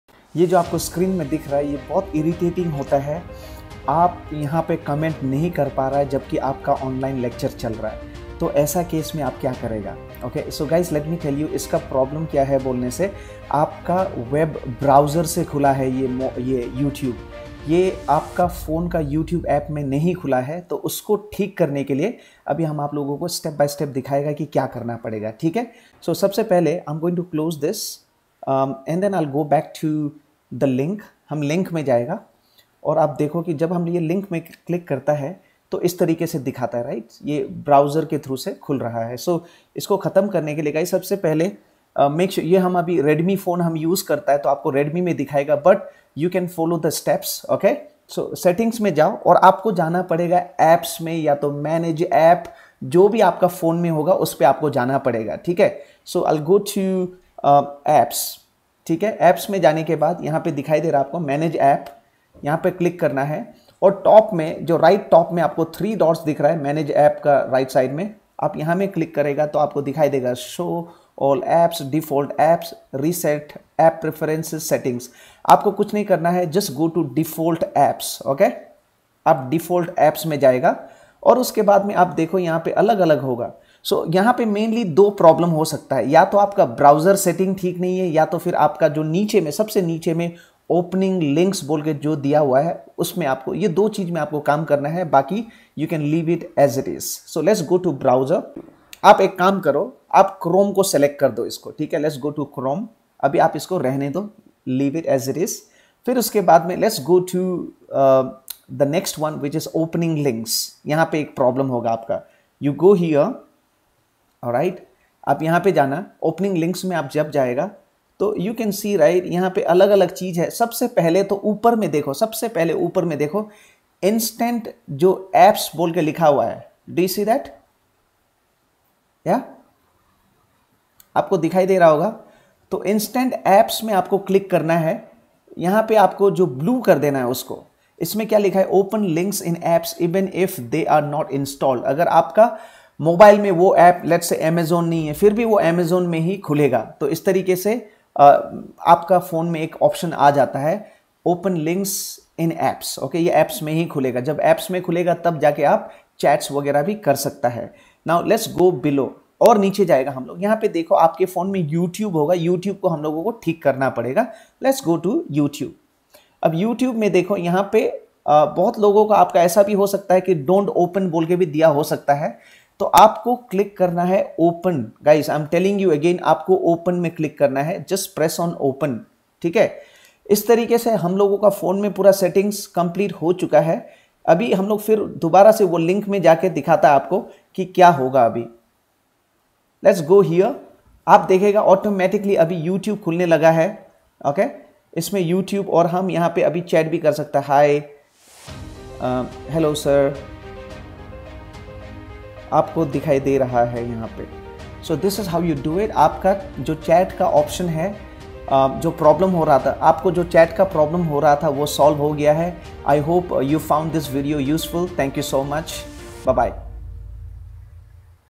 NOT ABLE TO CHAT ON YOUTUBE LIVE VIDEOS OR CLASSES/CHATTING PROBLEM ON YOUTUBE
In this tutorial video, you will learn how to do the proper settings on your phone to be able to CHAT on Youtube live videos or classes.